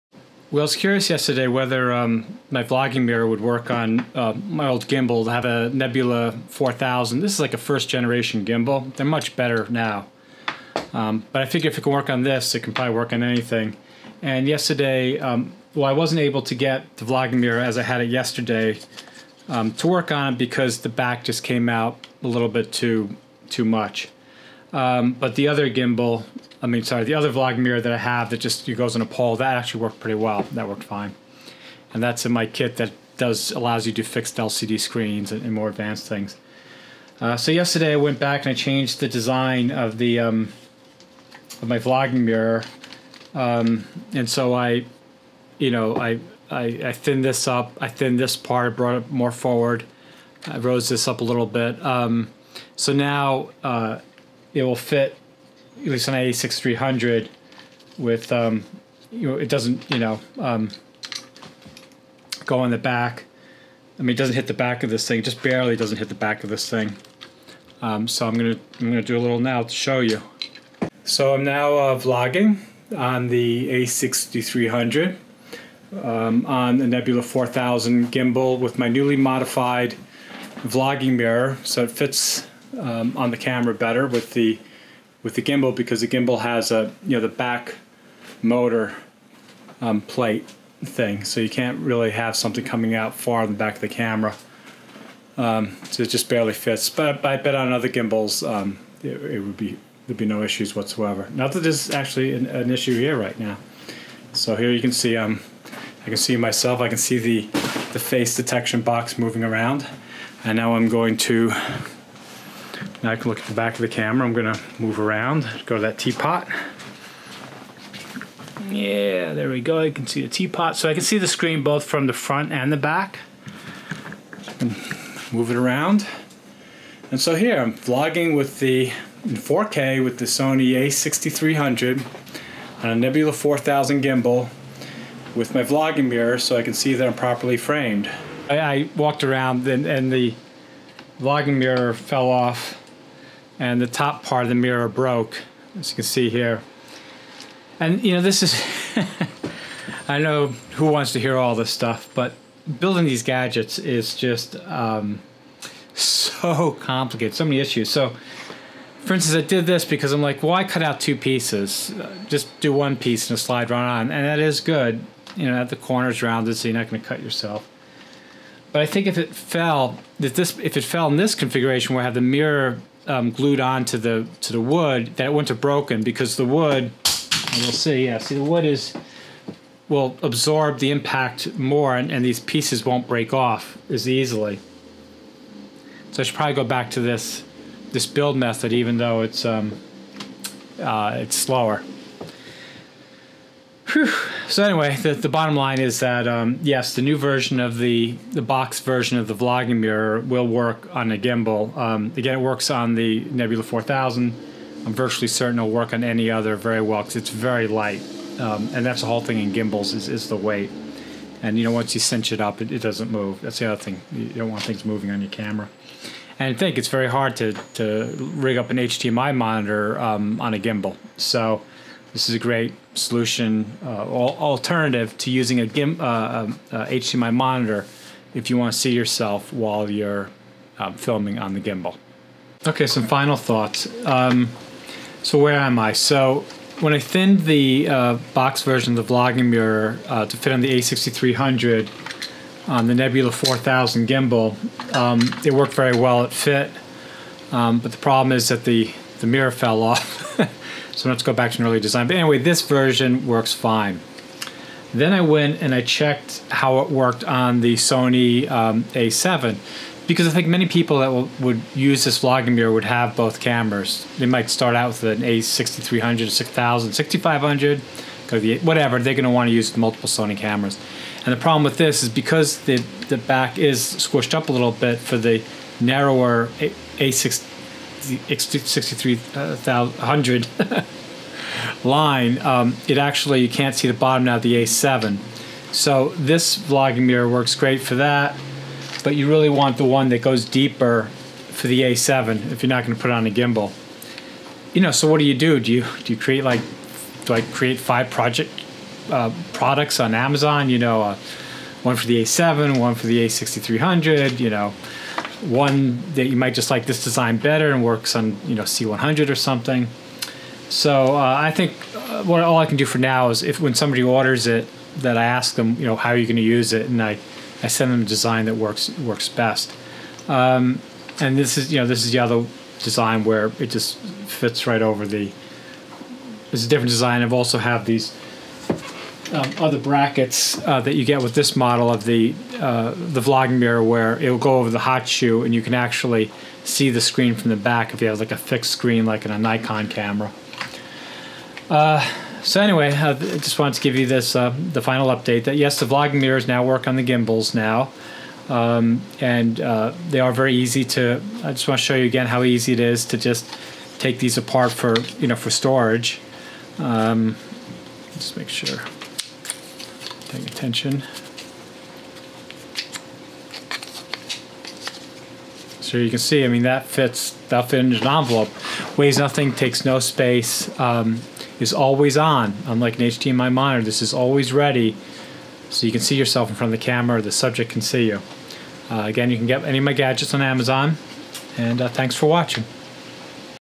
Gimbal + Sony Vlogging Mirror
After a few modifications the vlogging mirror (boxy version) works well with the Sony A6300 and Nebula 4000 Gimbal